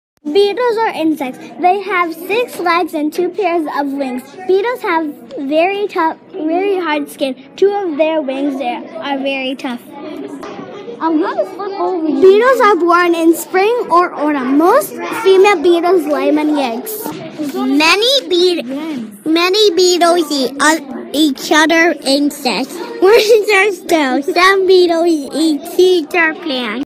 Beetles-Cunningham
2nd graders made bugs in art class and researched and recorded their information in library class.